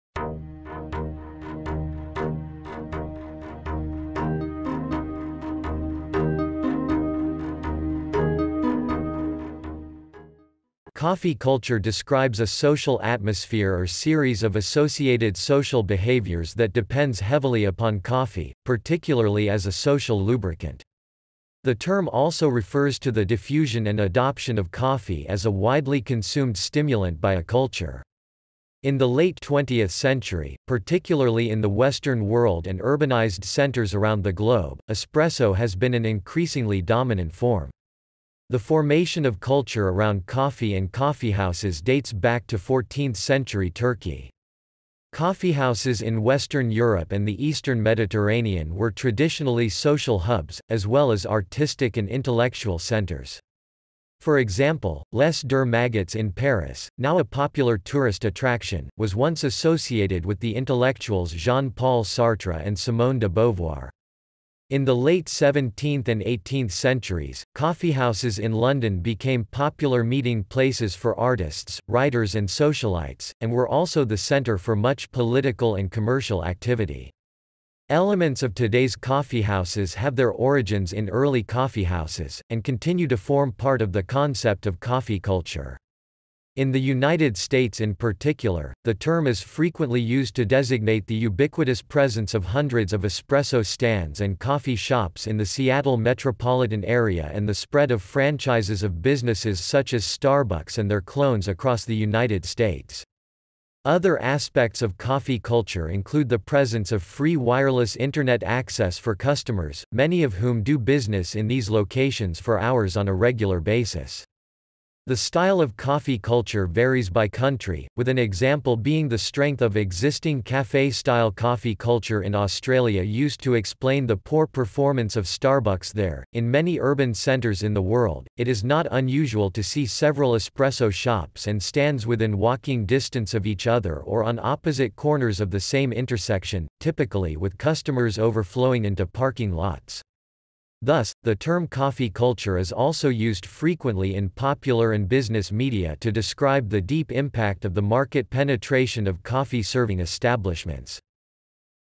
What is Coffee culture? Explain Coffee culture, Define Coffee culture, Meaning of Coffee culture
~~~ Coffee culture ~~~

Title: What is Coffee culture? Explain Coffee culture, Define Coffee culture, Meaning of Coffee culture

Description: Coffee culture describes a social atmosphere or series of associated social behaviors that depends heavily upon coffee, particularly as a social lubricant. The term also refers to the diffusion and adoption of coffee as a widely consumed stimulant by a culture. In the late 20th century, particularly in the Western world and urbanized centers around the globe, espresso has been an increasingly dominant form. The formation of culture around coffee and coffeehouses dates back to 14th century Turkey. Coffeehouses in Western Europe and the Eastern Mediterranean were traditionally social hubs, as well as artistic and intellectual centers. For example, Les Deux Magots in Paris, now a popular tourist attraction, was once associated with the intellectuals Jean-Paul Sartre and Simone de Beauvoir. In the late 17th and 18th centuries, coffeehouses in London became popular meeting places for artists, writers and socialites, and were also the center for much political and commercial activity. Elements of today's coffeehouses have their origins in early coffeehouses, and continue to form part of the concept of coffee culture. In the United States in particular, the term is frequently used to designate the ubiquitous presence of hundreds of espresso stands and coffee shops in the Seattle metropolitan area and the spread of franchises of businesses such as Starbucks and their clones across the United States. Other aspects of coffee culture include the presence of free wireless Internet access for customers, many of whom do business in these locations for hours on a regular basis. The style of coffee culture varies by country, with an example being the strength of existing cafe style coffee culture in Australia used to explain the poor performance of Starbucks there.In many urban centers in the world, it is not unusual to see several espresso shops and stands within walking distance of each other or on opposite corners of the same intersection, typically with customers overflowing into parking lots. Thus, the term coffee culture is also used frequently in popular and business media to describe the deep impact of the market penetration of coffee-serving establishments.